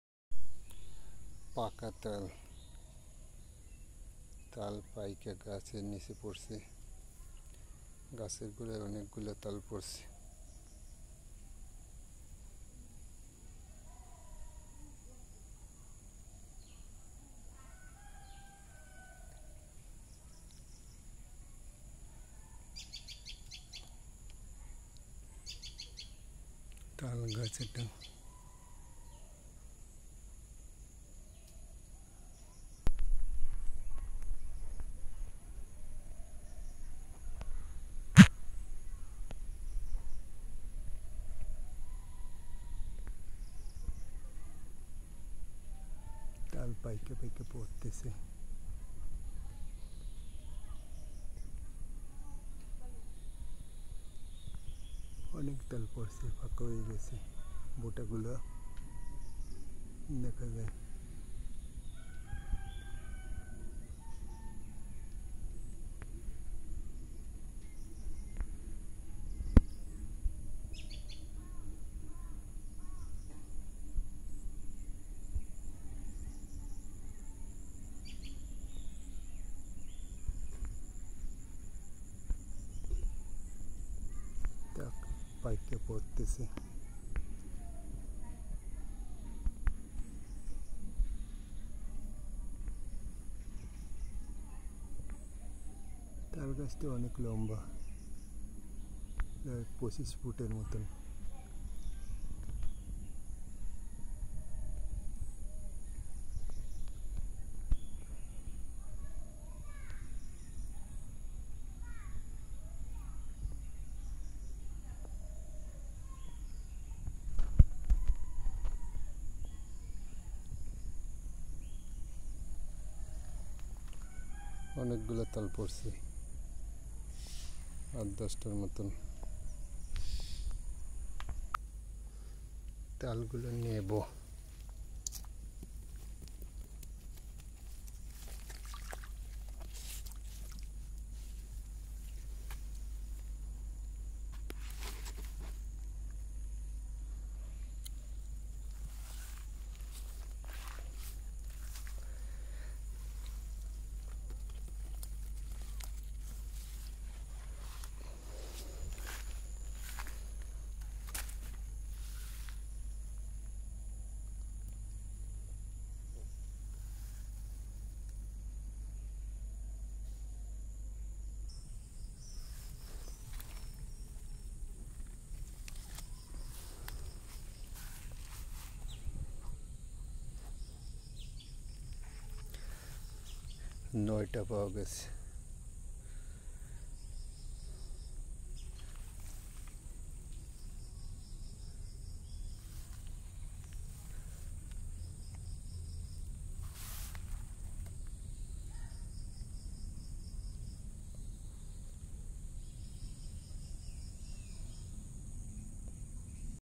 পাকা তাল পরছে। গাছ থেকে তাল পরছে
পাকা তাল পরছে। গাছ থেকে তাল পরছে। গাছ তলায় তাল পরছে। তাল পেকে পরছে গাছের  ঘোরাতে